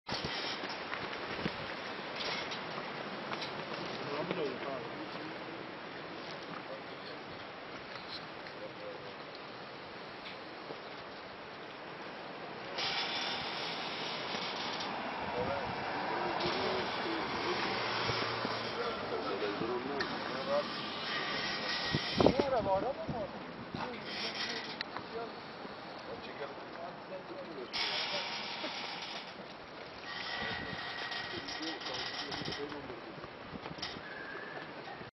DEREKÖY'DE HAPLARI BIRAKIP KAÇTILAR, KAPIKULE'DE YAKALANDILAR 30/01/2014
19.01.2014 tarihinde Trakya Gümrük ve Ticaret Bölge Müdürlüğü bağlantısı Dereköy Gümrük Kapısında yapılan saha devriyesinde poşetler içerisinde yere atılmış 33 kilo 265 gram (82.550 adet) hap tespit edilmiştir. Kamera Sisteminden yapılan tarama sonucunda, hapların Bulgaristan uyruklu A.H.C. isimli şahsın sürücülüğünü yaptığı yabancı plakalı araç ile Bulgaristan uyruklu D.A.D.isimli şahsın sürücülüğünü yaptığı ve içerisinde I.T.K. ve I.H.M. isimli şahısların yolcu olarak bulunduğu yabancı plakalı araçlar tarafından atıldığı tespit edilmiş ve söz konusu araçlar ihbar kayıtlarına alınmıştır.